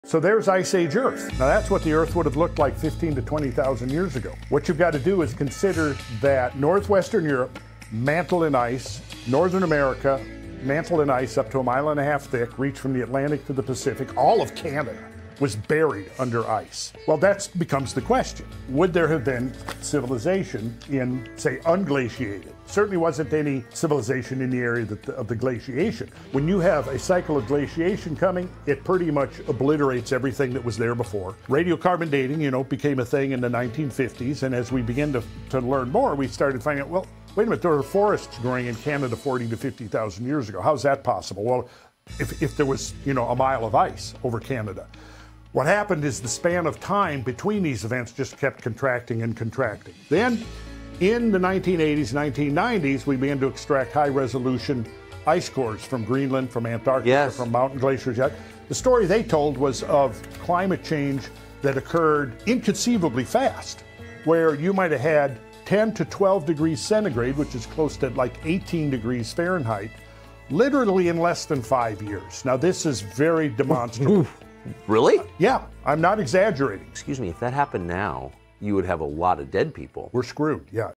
The Ice Age Wasn’t Slow
15,000 years ago, Canada was buried under 1.5 miles of ice. Civilization? Obliterated.

But the shocker came later—ice cores revealed temperature swings of 18°F in less than 5 years.

Climate didn’t shift gradually… it snapped. And if that happened today?

We’d be in serious trouble.